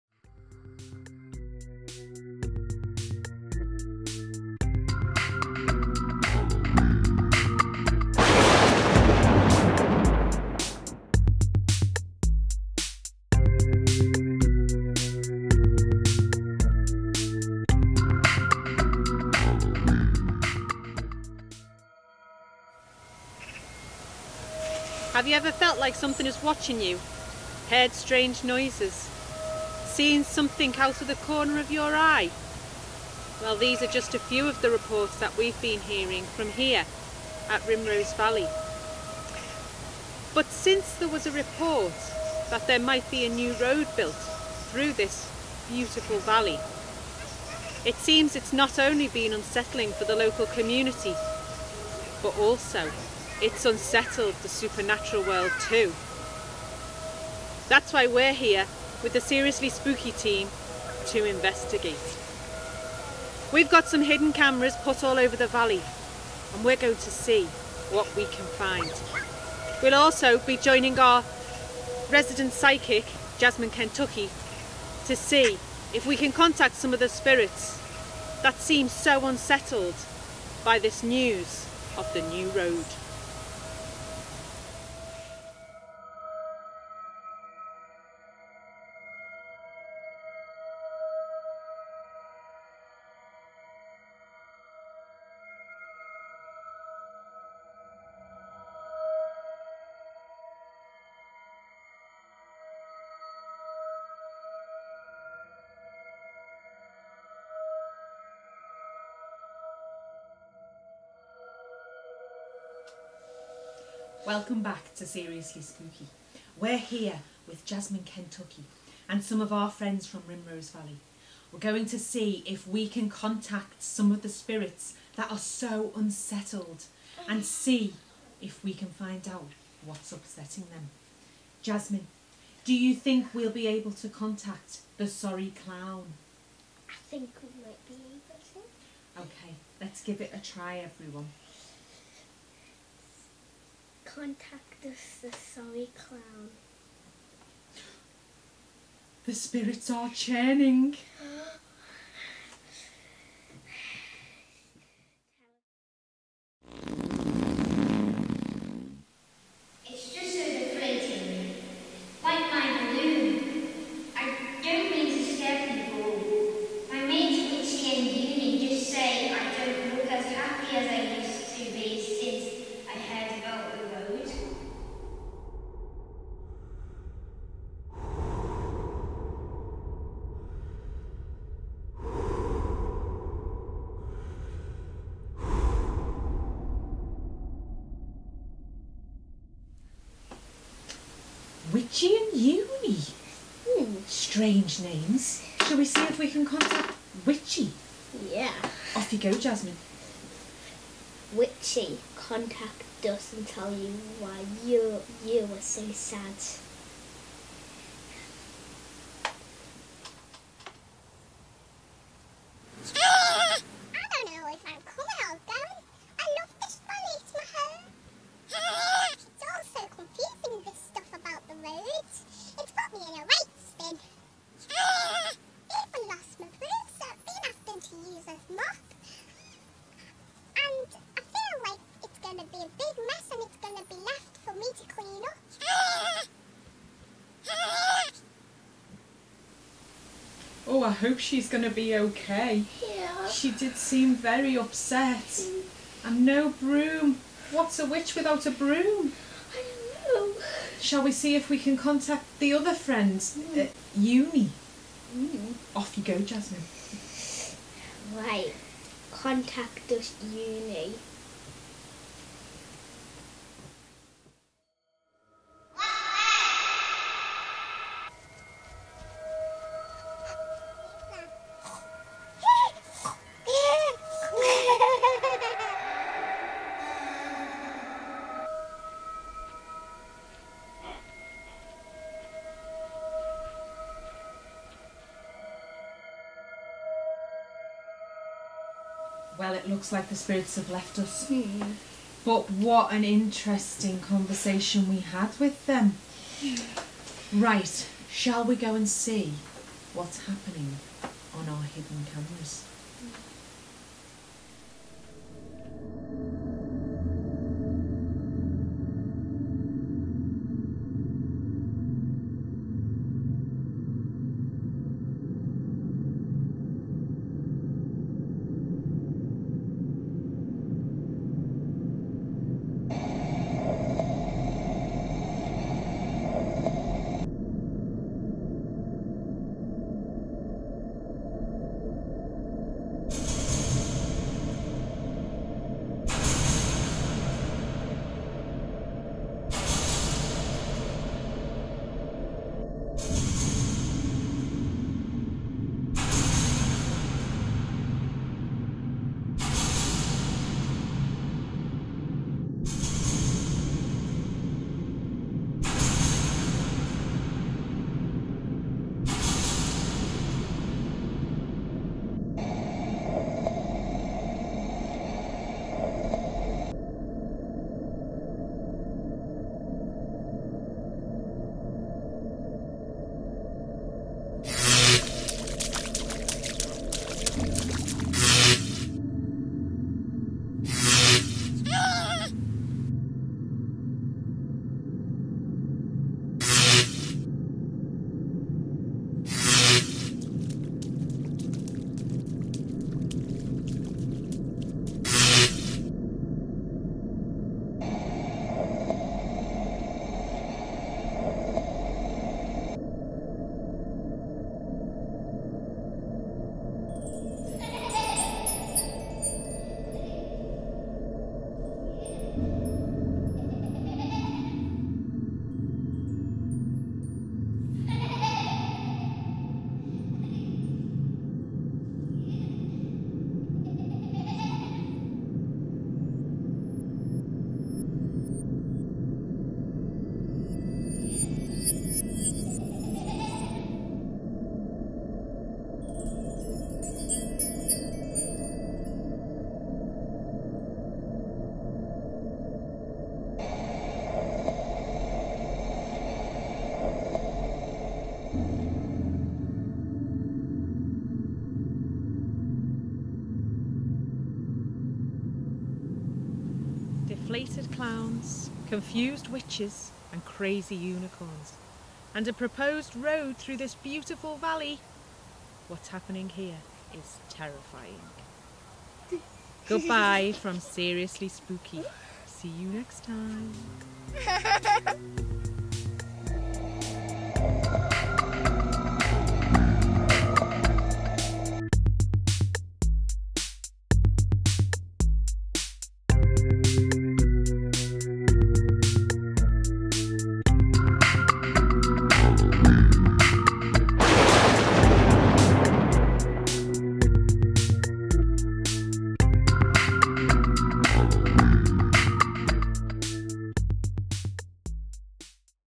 Seriously Spooky on Rimrose Valley
It seems that the news about a new road being built hasn't just unsettled the local community... The Seriously Spooky team investigate some supernatural goings on at Rimrose Valley.